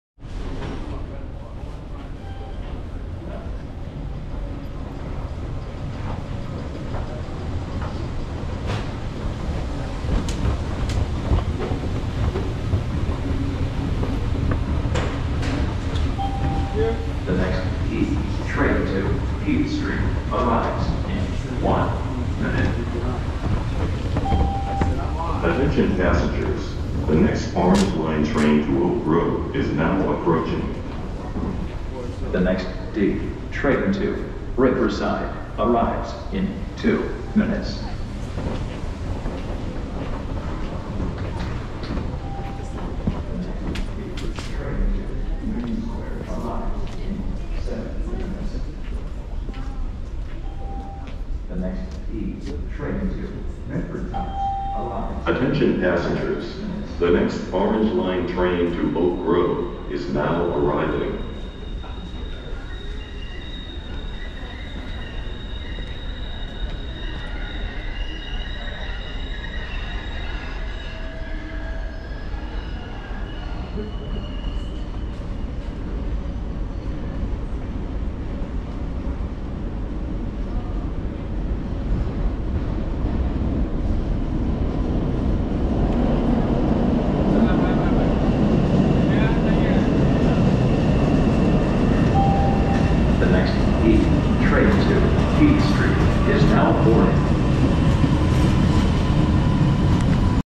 North Station Boston 2x Trains - Orange and Green Line MBTA Tour, Map, How To Ride🟠🟢
Take a walking tour of North Station in Boston, where you can access the Orange and Green Lines. Learn how to ride the T and explore the area with our helpful map guide! 🟠🟢